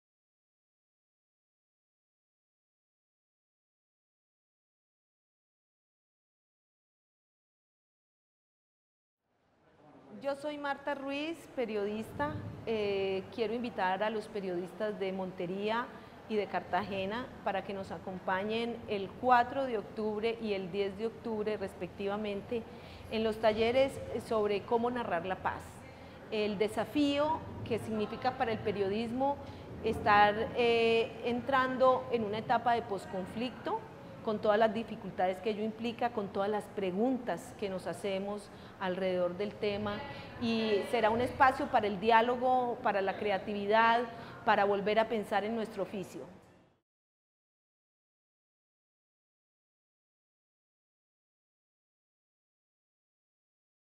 Invitación a participar en taller ¿Cómo narrar la paz?
Marta Ruiz, periodista y asesora editorial de Revista Semana, invita a personas que comparten su oficio en Montería (octubre 4) y Cartagena (octubre 10), a que se inscriban para participar del seminario-taller ¿Cómo narrar la paz?